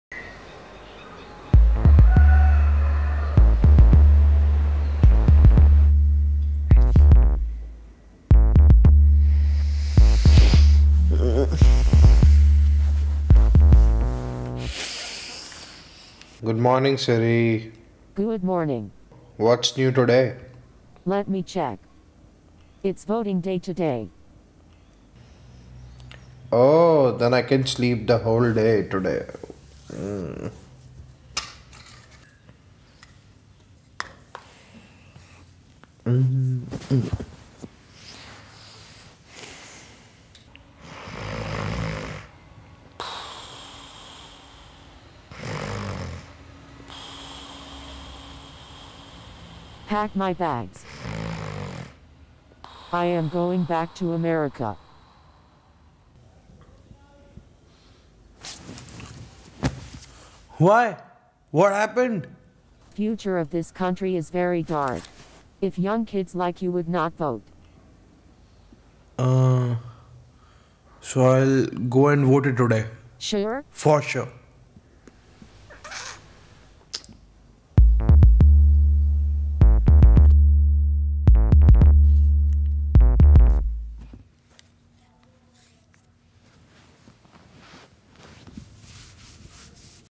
iVote
An comical parody which inspires young generation to vote.